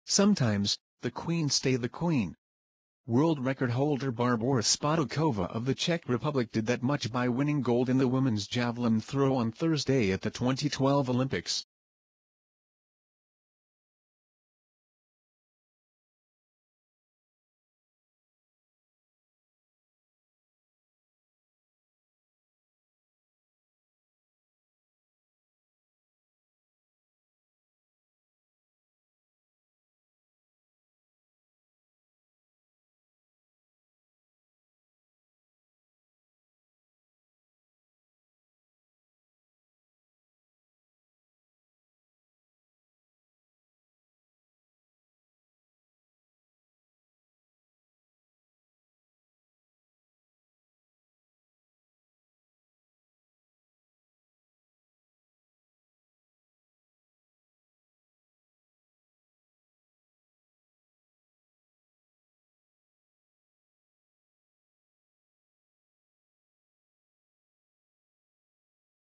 Barbora Spotakova Wins Women's Javelin Throw Final Gold Medal 2012 London Olympics
Sometimes, the queen stay the queen. World record holder Barbora Spotakova of the Czech Republic did that much by winning gold in the women's javelin throw on Thursday at the 2012 Olympics.

Spotakova hurled the javelin 69.55 meters on her fourth throw, giving her an easy win in the final over Christina Obergfoll of Germany, who mustered 65.16 meters on her one throw in bounds. All four of Spotakova's in-bounds attempts were better than Obergfoll's, and good enough for gold.Spotakova didn't threaten her world record of 72.28 meters, but she repeated as the gold medalist in the event, becoming only the second woman to do so, and first since Ruth Fuchs went back-to-back in 1972 and 1976. Obergfoll's silver followed a bronze in the event in Brazil.Germany's Linda Stahl won bronze with a throw of 64.91 meters.